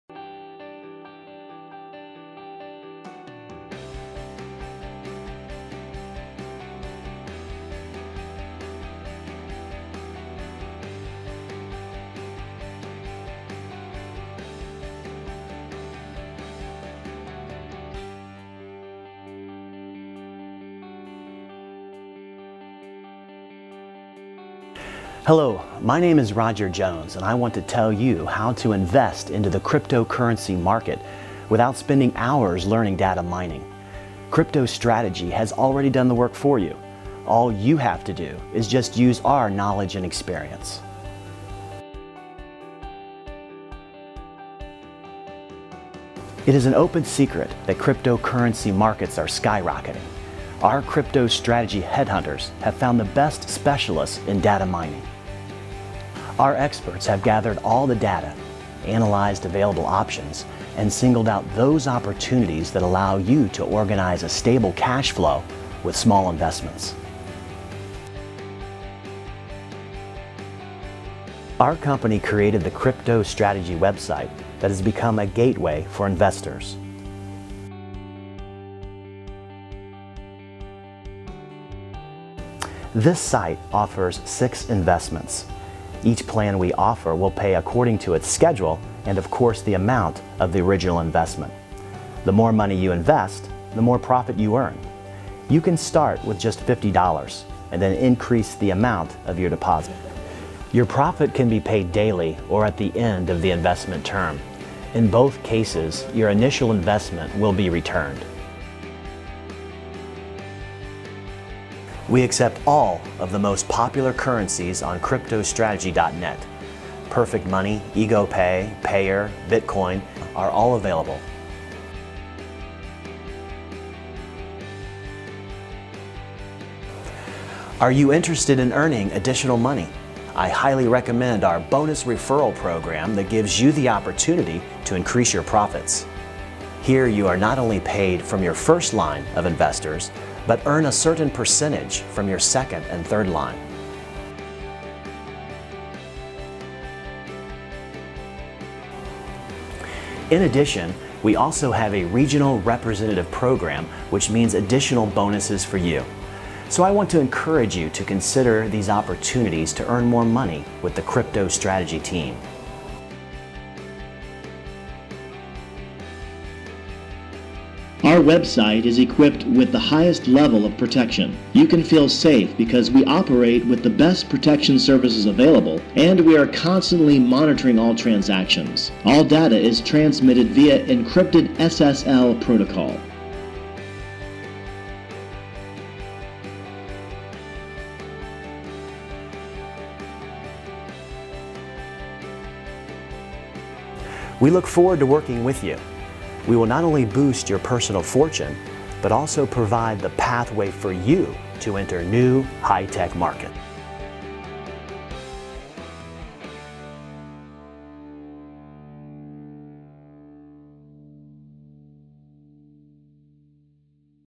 CryptoStrategy.Net - your reliable crypto investments!
Cryptocurrencies are a real economic breakthrough that revolutionized the world of electronic currencies, and formed a unique and invulnerable monetary system. Millions of people around the world are engaged in the improvement of digital currencies, progressive mining, and investing in cryptocurrencies. CryptoStrategy Limited makes successful attempts to unite the efforts of enthusiasts in the field of cryptocurrencies mining. The company also wants to create favourable conditions for the earnings of people who are far from computer technology, but who constantly try following the innovative processes and extract economic benefits.